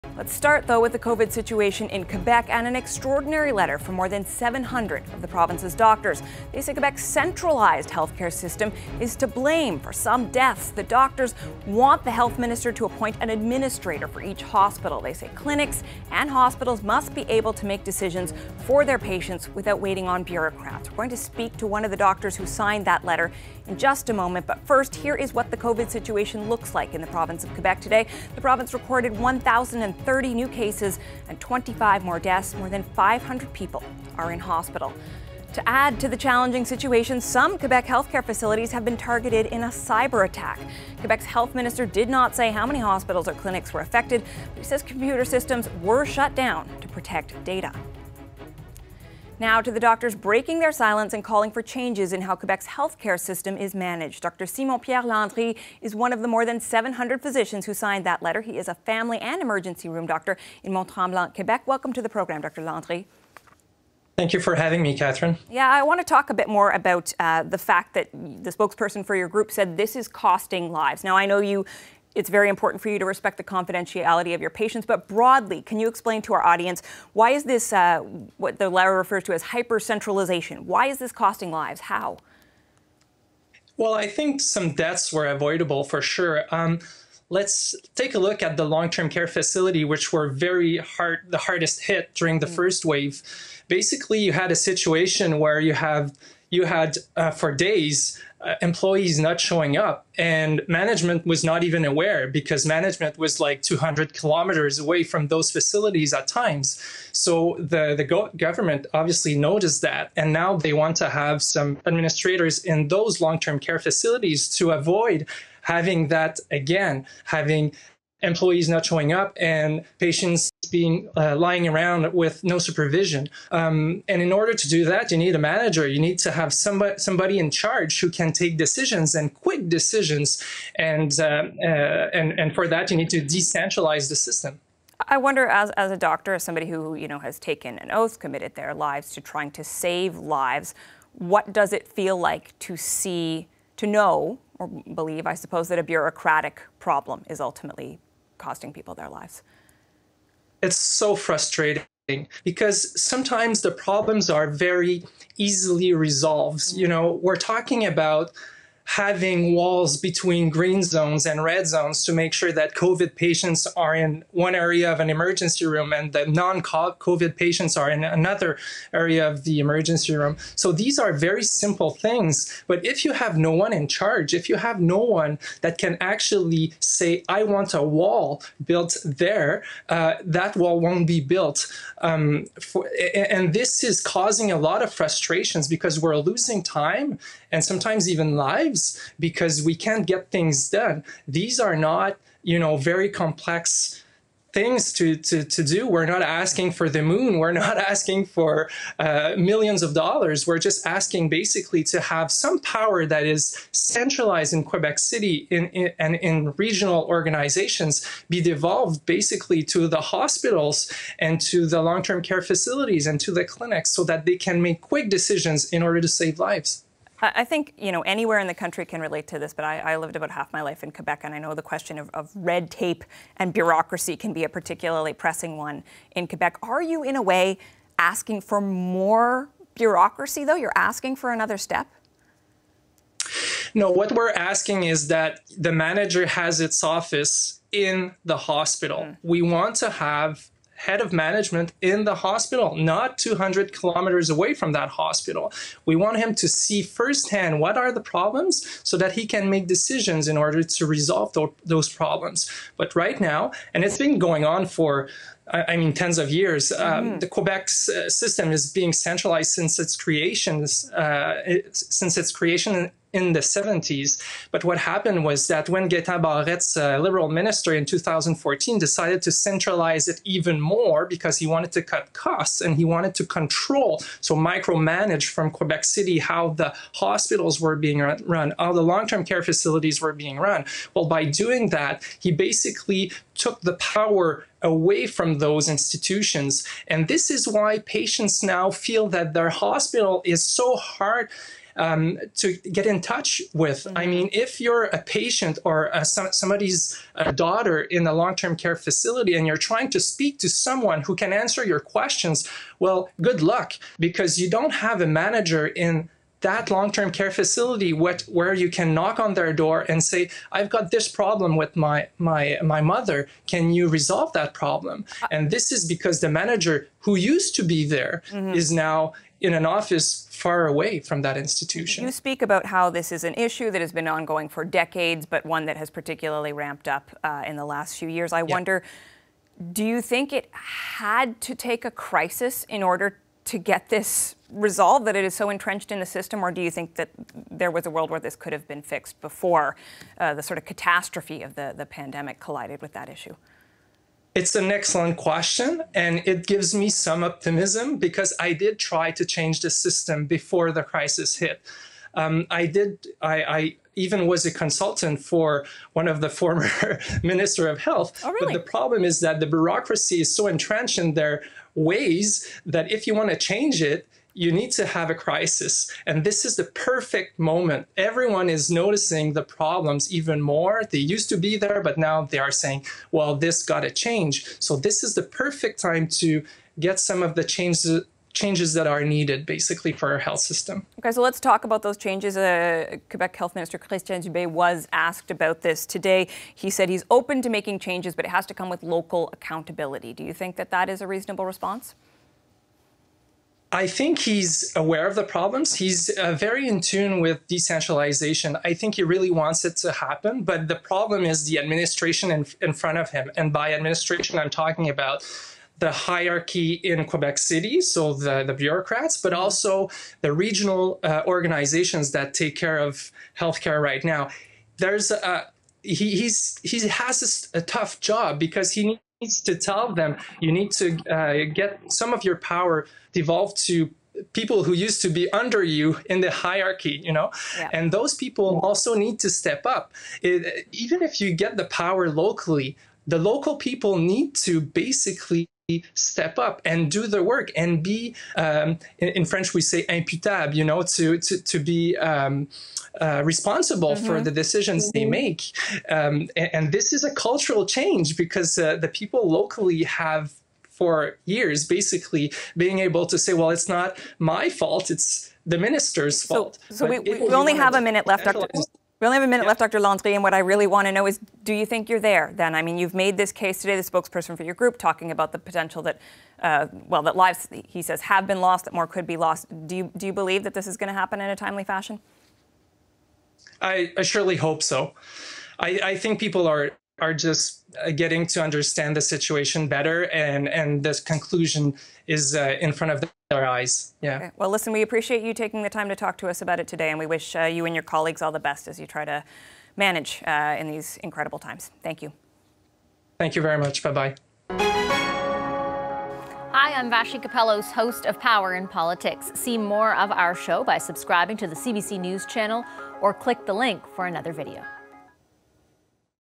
Centralization led to many deaths during pandemic, say Quebec doctors
Seven hundred Quebec doctors signed a letter to the minister of health arguing that centralized decision making has caused too many deaths during the COVID-19 pandemic.